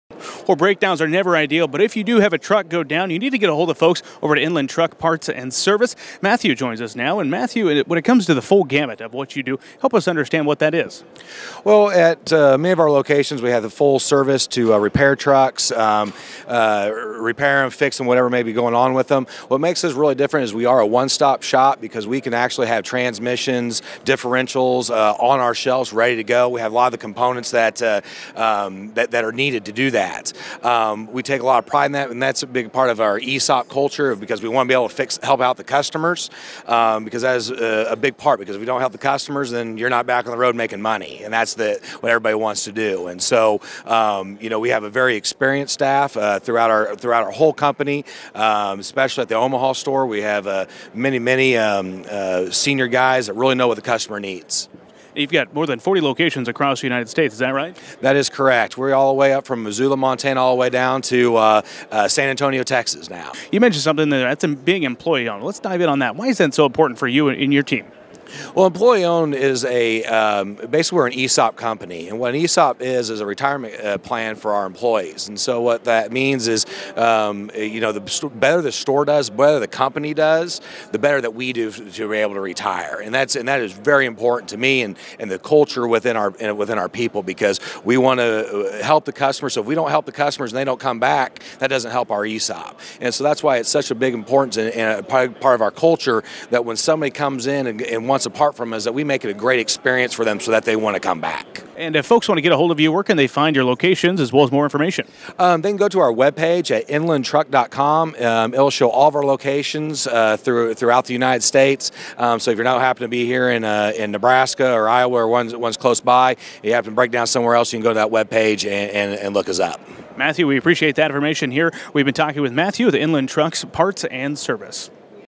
Inland Truck Parts & Service
Being employee-owned is part of the reason Inland Truck Parts & Service is successful.

Learn more about the services Inland Truck Parts & Service provides!